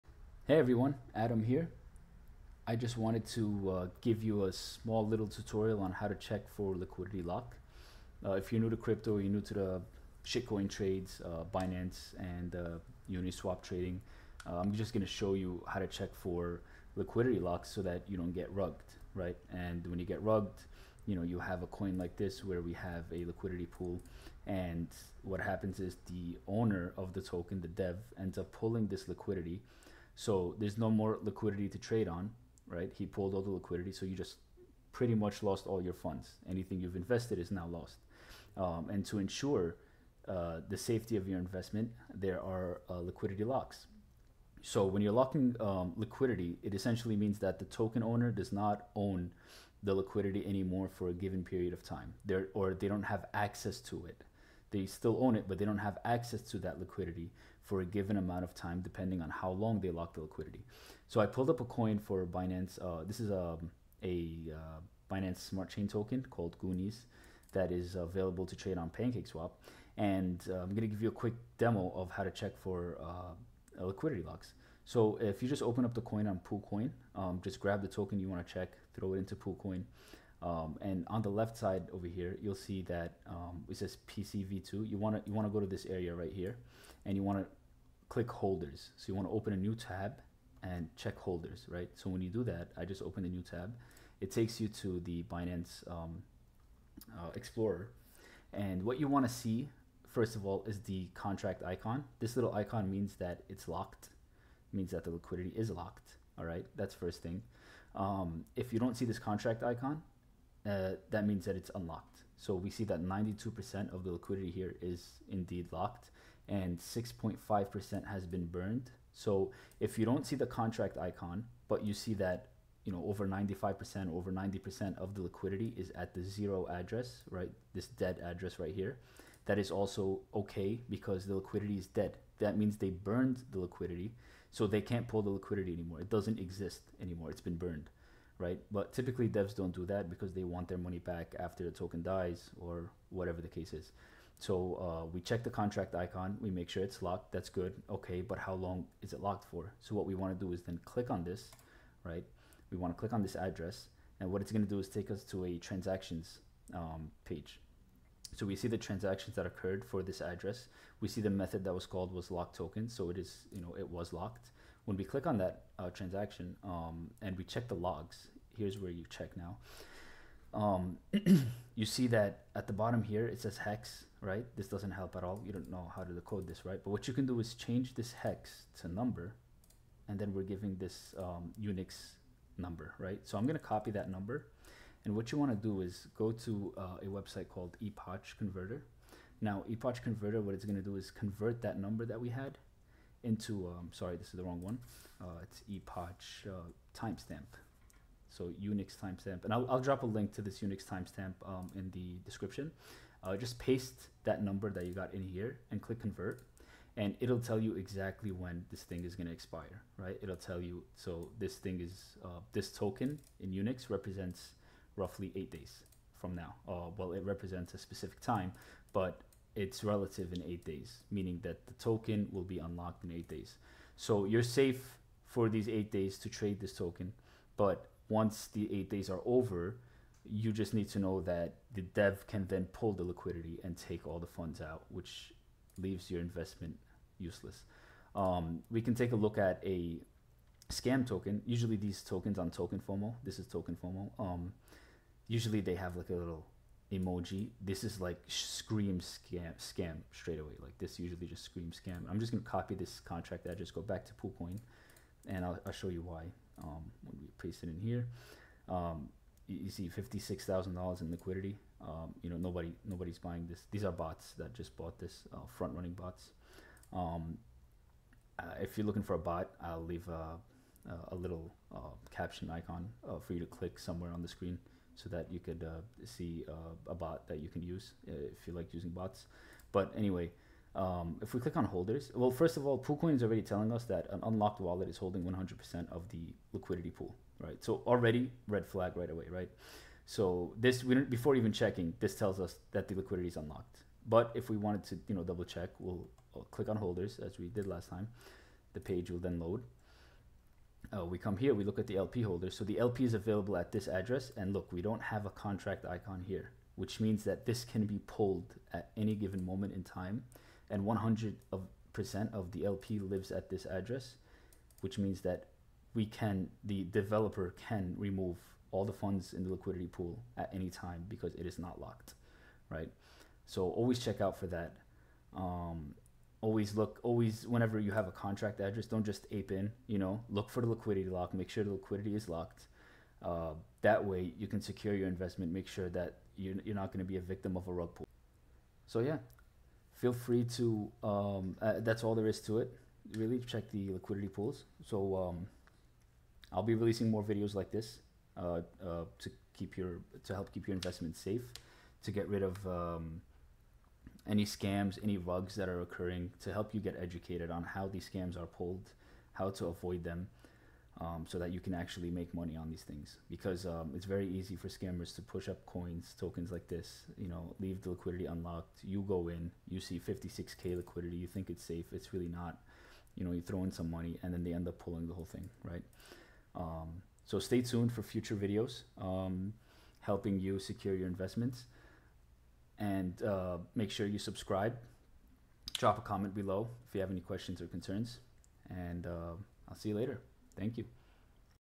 How To Check Liquidity Lock - Binance Smart Chain
Check for liquidity locked isn't as straightforward for beginners who are new to the binance smart chain space. It can be painful to have to wait for token owners to drop a liquidity locked link without you knowing for sure whether or not the liquidity is truly locked.

In this video, I show you how to find how much liquidity is in the liquidity pool, as well as how to check for liquidity lock, and for how long that liquidity is locked for.

Liquidity gets added through pancakeswap, and also pulled from pancakeswap as well. If you want to learn what is liquidity and how to check if it's locked, then this video is for you.

liquidity locked, check for liquidity lock, binance smart chain, liquidity pool